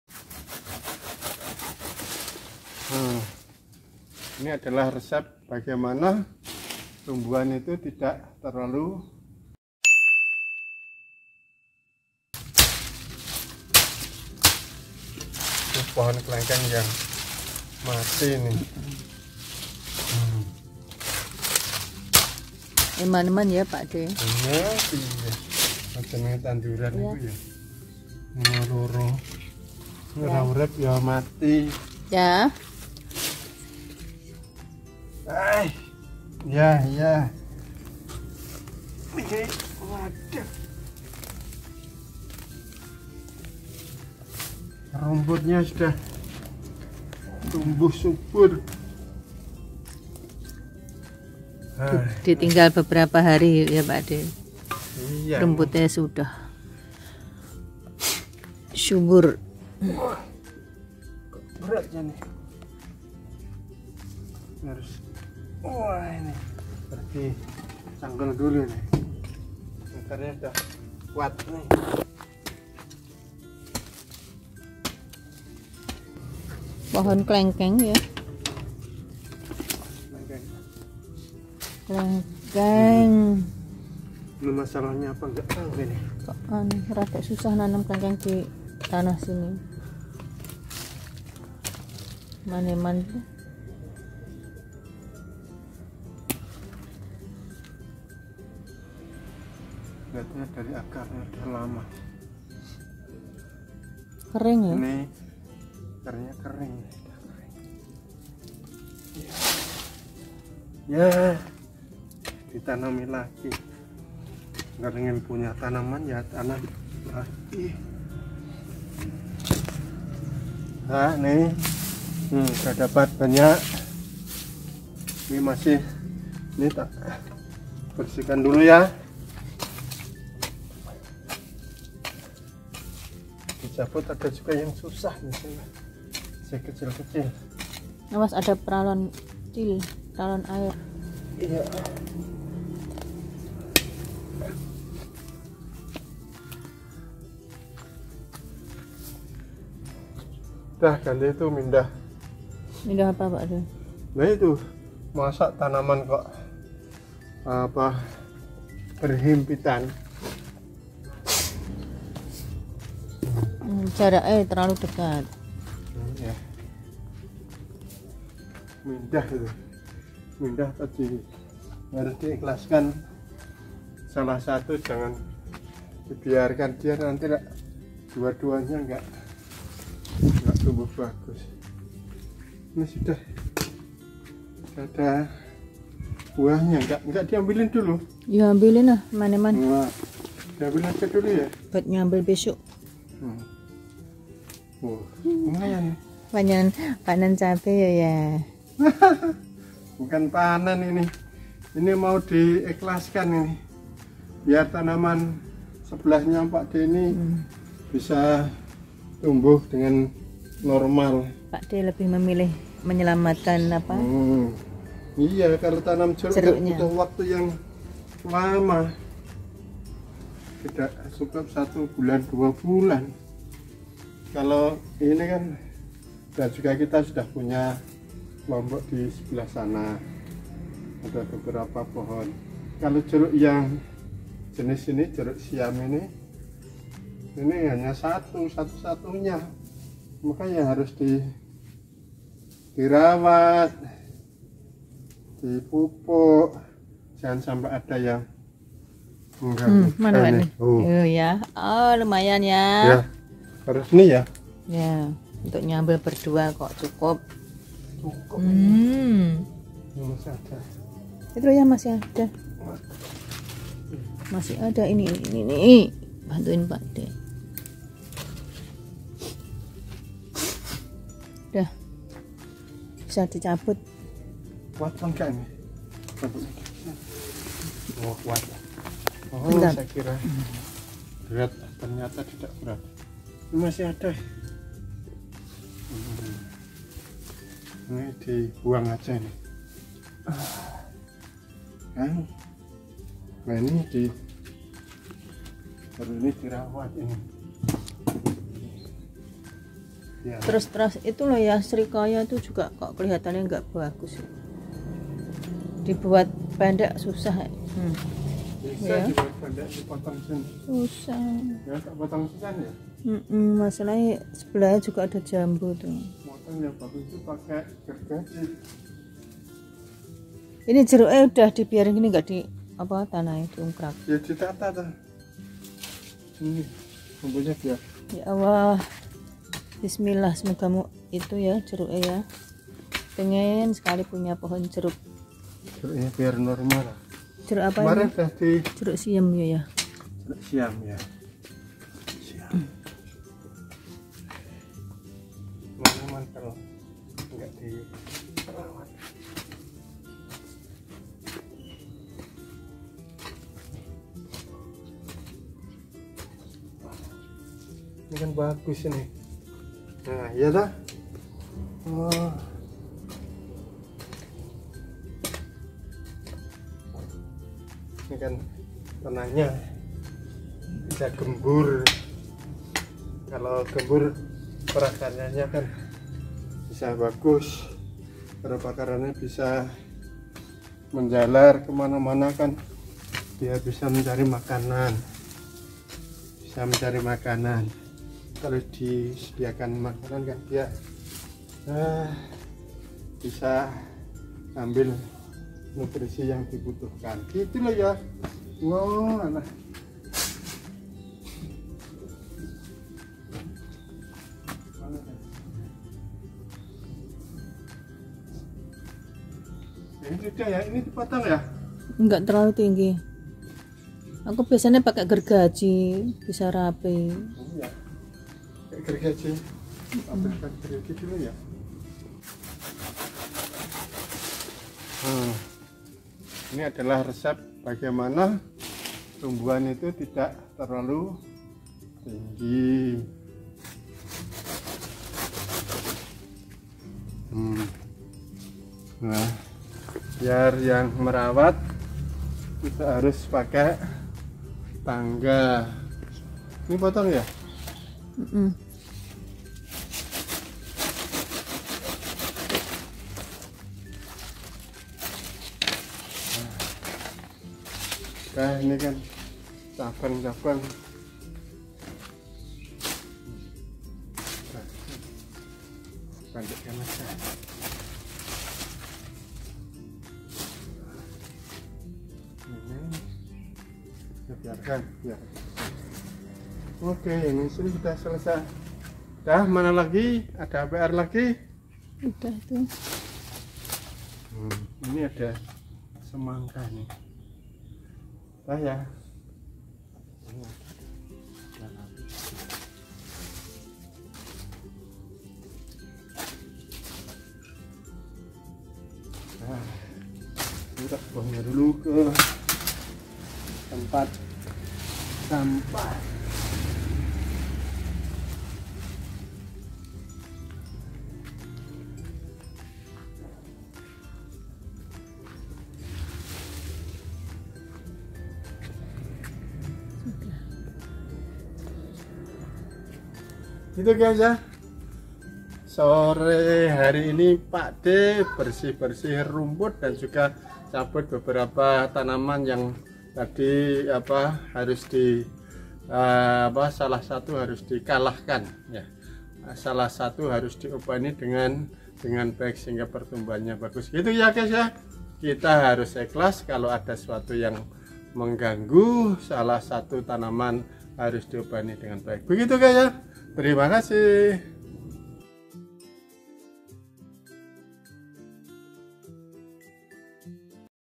Pohon Kelengkengnya Pakdhe Mati Gesss!
Dalam video ini menceritakan tentang kegiatan sehari-hari pasca pensiun seseorang yang bernama M. Djupri. Beliau adalah pensiunan dari Kepala Sekolah SMA.